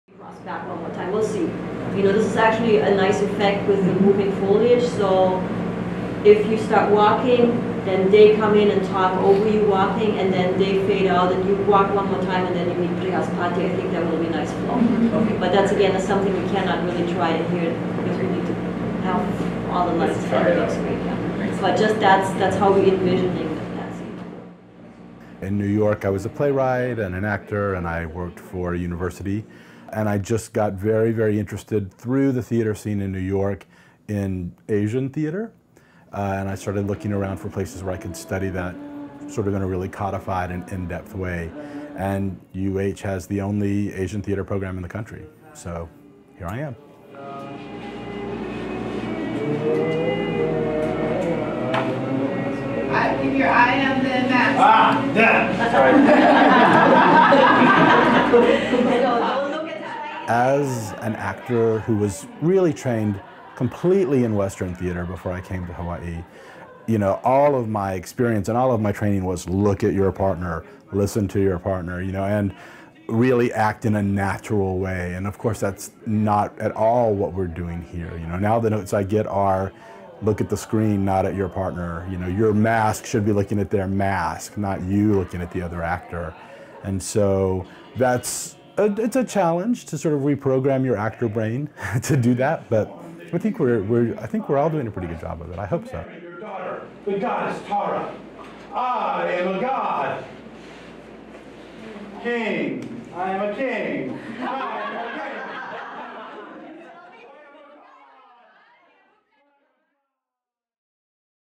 Episode 15: Enriching the actor's toolkit by training in different forms
"As an actor who was completely trained in Western theatre before coming to Hawai‘i...it's a challenge to re-program your actor brain." - Matthew Kelty, Shadow Actor

* To watch the entire documentary in one sitting, go here and click “Watch”